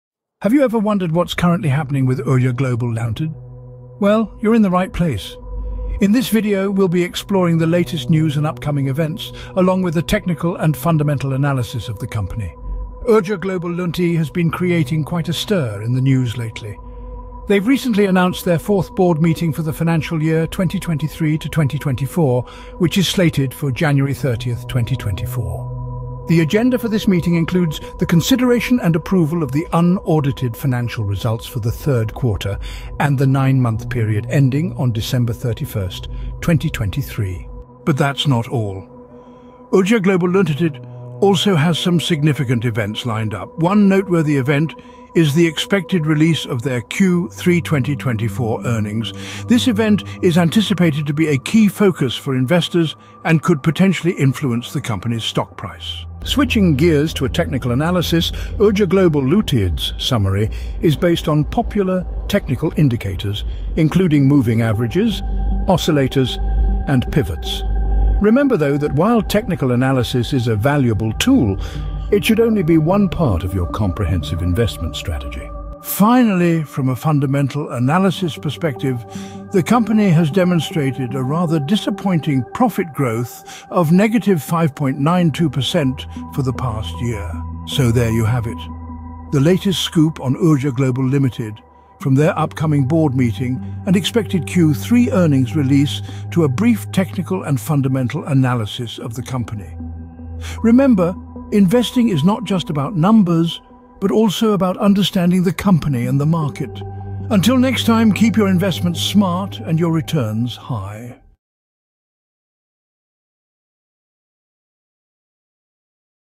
Urja Global Ltd Latest News and Analysis | Fundcode
Whether you're a seasoned investor or just getting started, this video is packed with valuable insights that will help you make informed decisions about your investments. Let's dive into the world of Urja Global Ltd together and uncover its potential!

OUTLINE:

00:00:00 Introduction and Board Meeting Announcement
00:00:37 Q3 Earnings Release
00:00:56 Technical Analysis
00:01:15 Fundamental Analysis
00:01:27 Conclusion